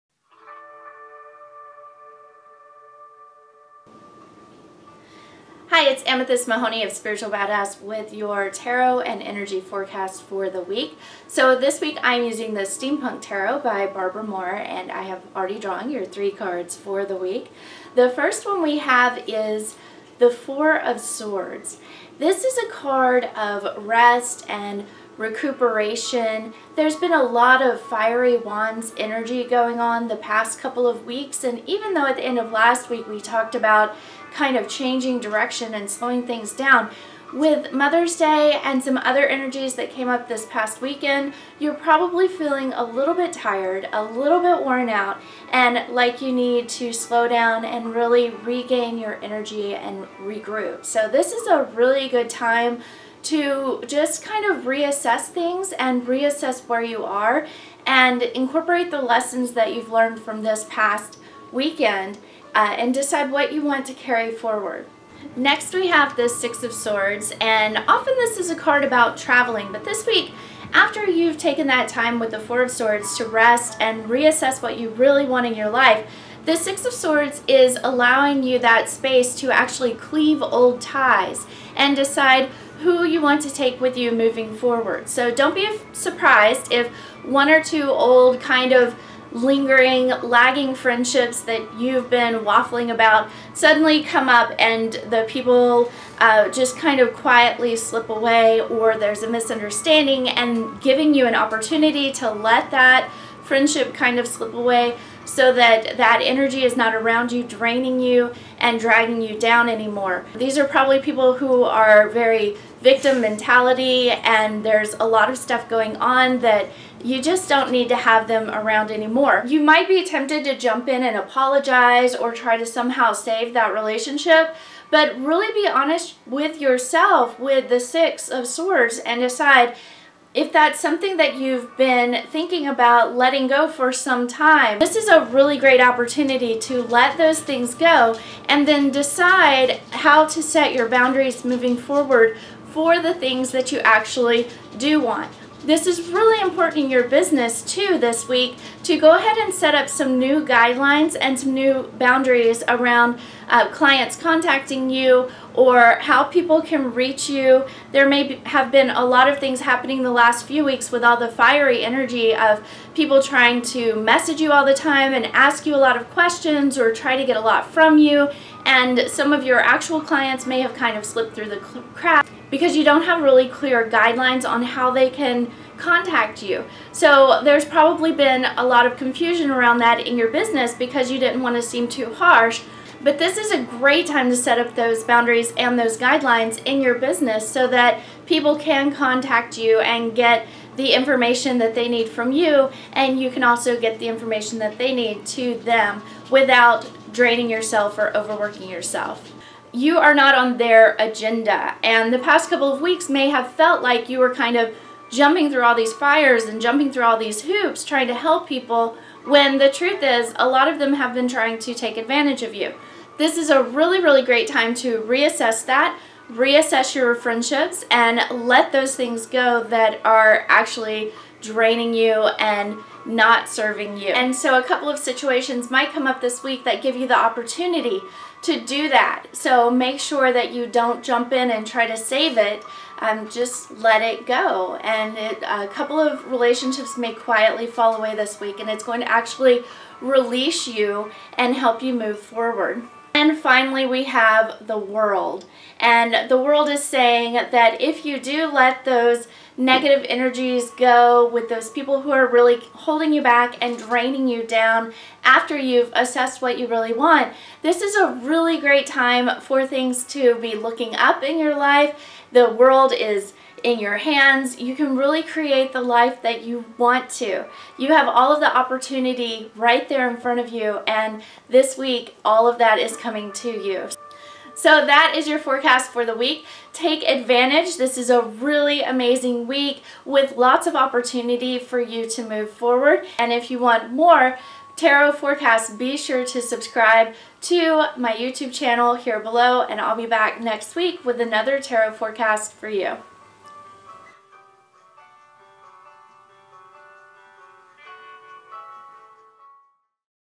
Tarot and Energy Forecast May 11-17
Tarot and Energy Forecast May 11-17 with the Steampunk Tarot by Barbara Moore.

This week's cards:

4 of Swords - It's time to rest from all of the fiery energy of the past few weeks, plus Mother's Day. Take some time to decide what you really want moving forward.

6 of Swords - Cleave old relationships that no longer serve you and set new boundaries as you move ahead.

The World - If you've followed the advice from the first two cards, things should be looking up. You have great opportunities ahead of you, but only if you let go of the past and negative energy that was hanging around.